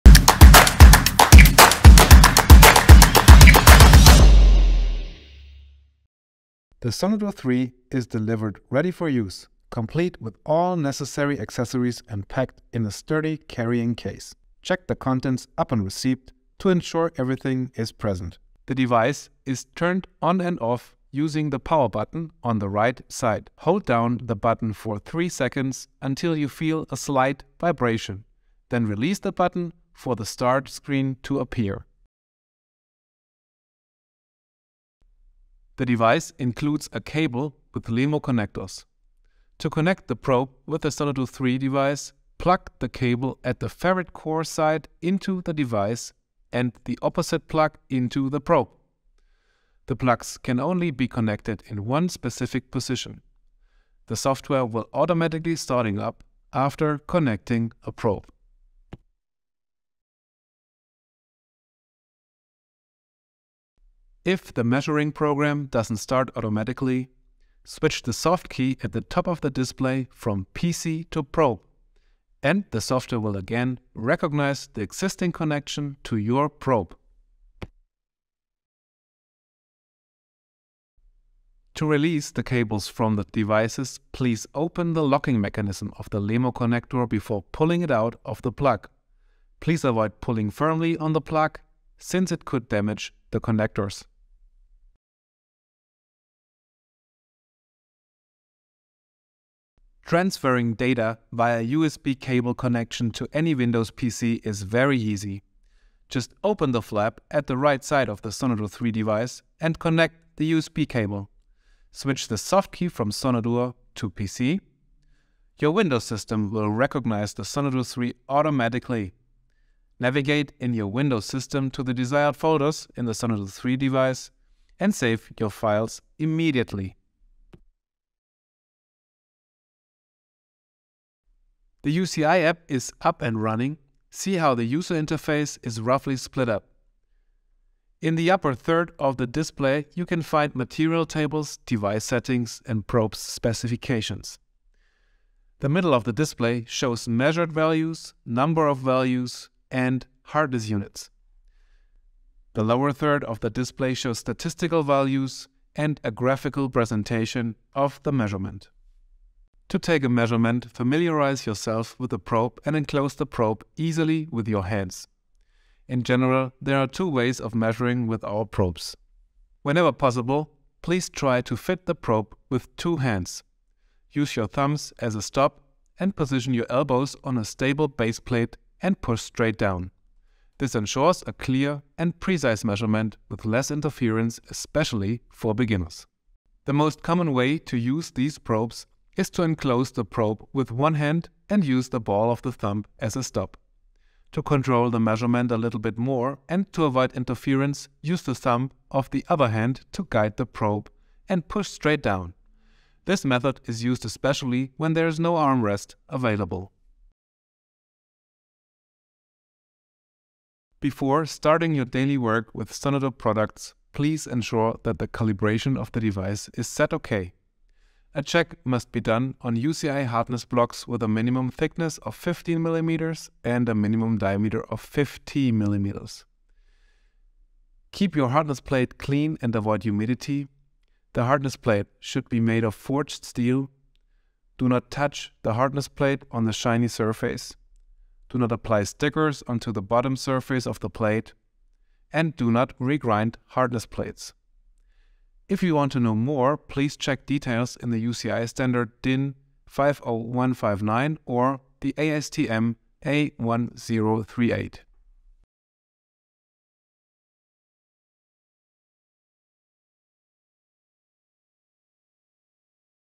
SONODUR 3 - Start-up
The scope of delivery of the SONODUR 3 hardness tester includes many features. In this video we show the most important functions for the initial start-up.

Do you have recommendations for further videos? Please write your suggestions in the comments.

The portable hardness testing is usually carried out directly on-site on components with mobile devices, which also allows an initial evaluation of the results. The great advantage of portable measuring technology is:
- Easily movable for fast and selective decision making on site.
- Can be used where classic hardness testing (Vickers, Rockwell, Brinell) is not possible or impractical, such as in the area of incoming goods inspection, production control or maintenance on integrated components.

Timestamps:
00:00 Intro
00:08 Scope of delivery
00:18 Power on
00:35 Connecting SONODUR 3 & Probe
01:03 USB controller setting
01:21 Safety instructions for unplugging the probe
01:41 Connecting SONODUR 3 with a PC
02:11 SONODUR 3 GUI overview
02:39 How to measure with SONO Probes
03:31 Calibration